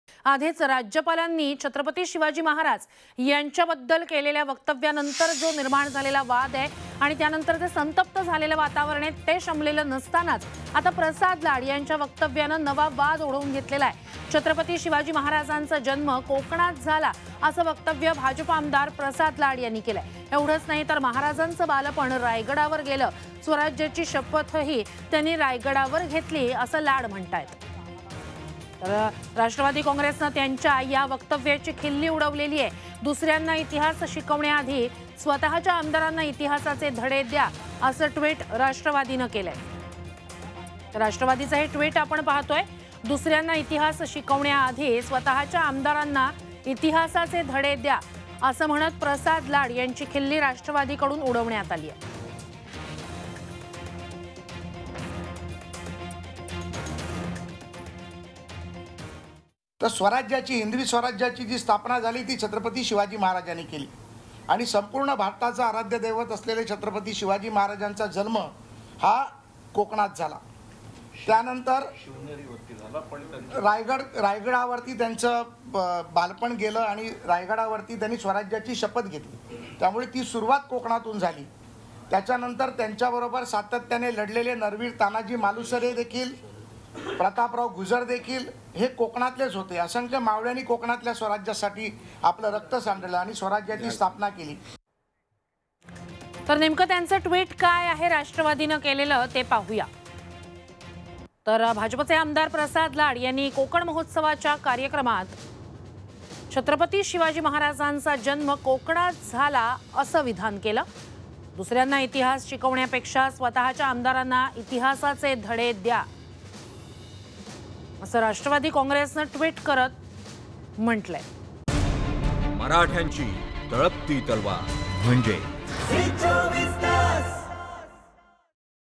Prasad Lad म्हणाले शिवाजी महाराज कोकणात जन्मले! NCPने केली तीव्र टीका | zee 24 taas
Maharashtra News Updates | Marathi News LIVE : एका क्लिकवर दिवसभरातील बातम्या
Social Media Links :
5 Minitat 25 Batmya । 5 मिनिटांत 25 बातम्या